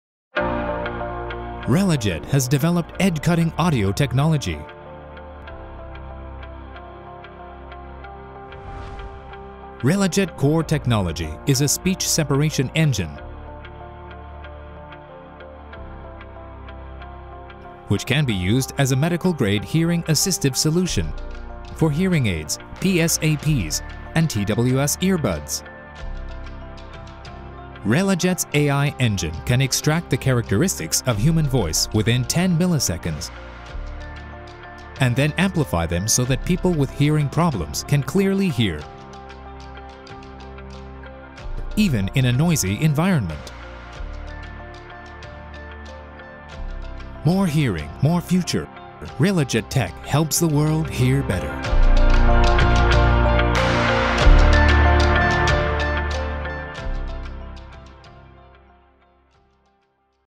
More hearing, more future, RelaJet Tech helps the world hear better.
Otoadd is a product with innovative AI technology to enhance the audio quality and overall product practicality.
Checkout TTA Team at CES2021